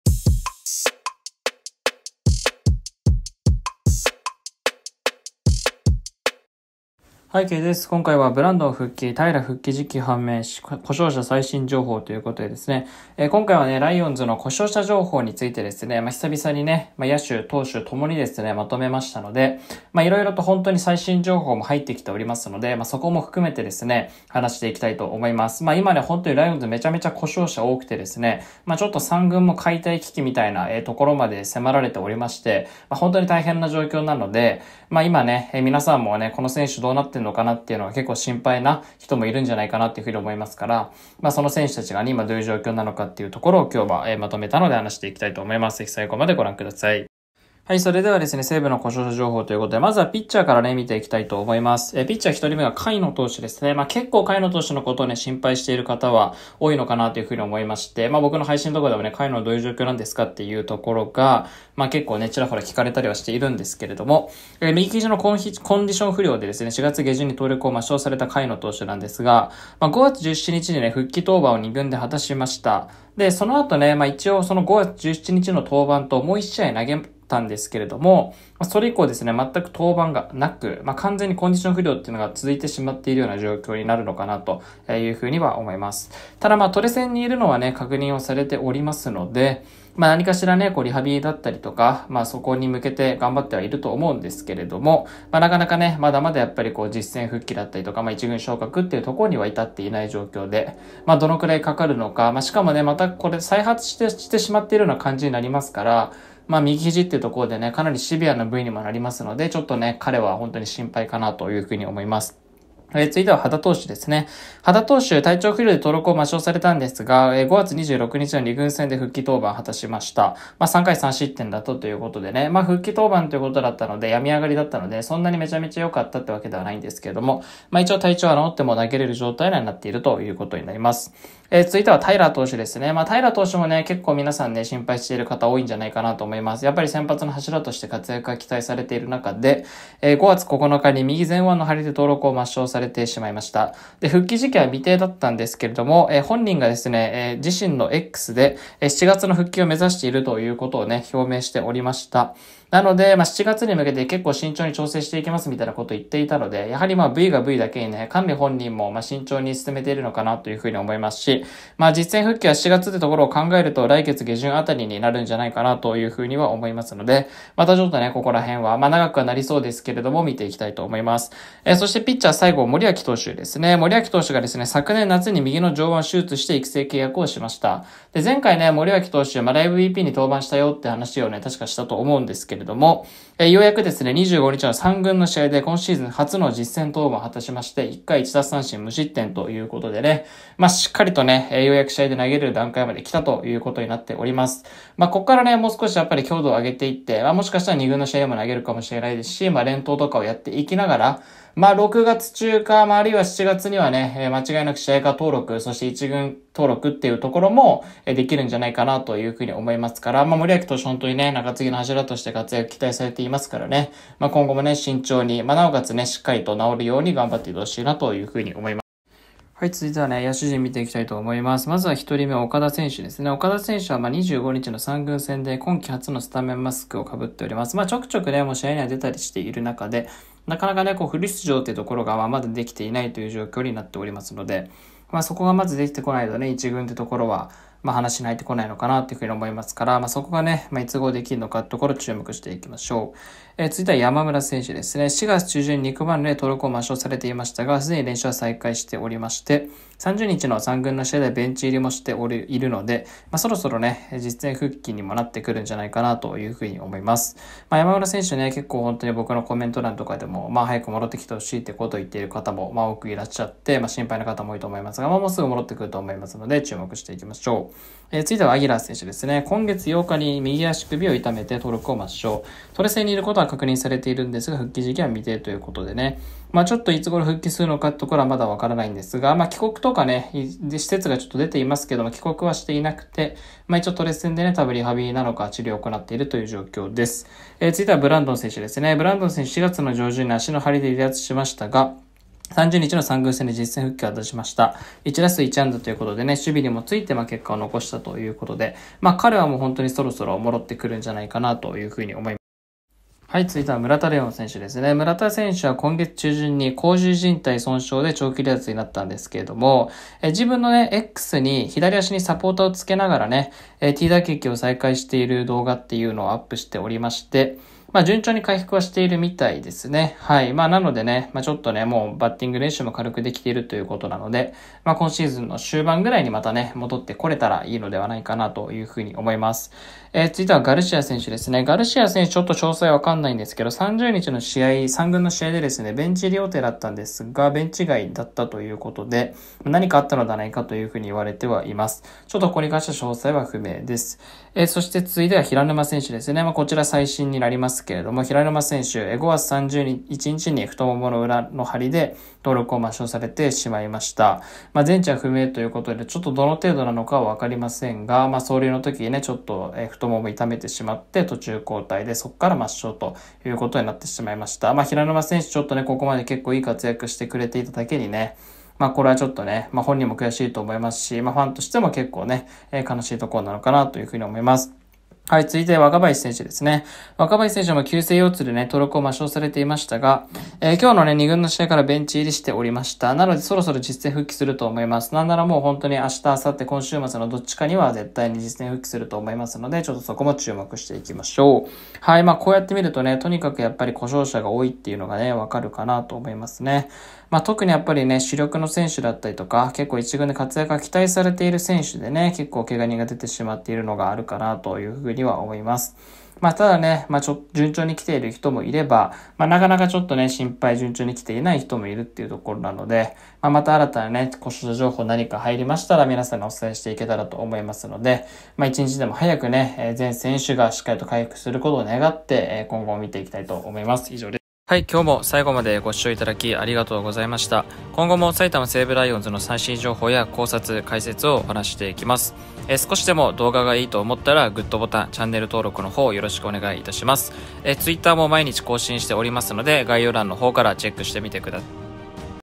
【西武】マジで故障者ばかり…故障者最新情報
今回は西武の故障者情報について話していきます！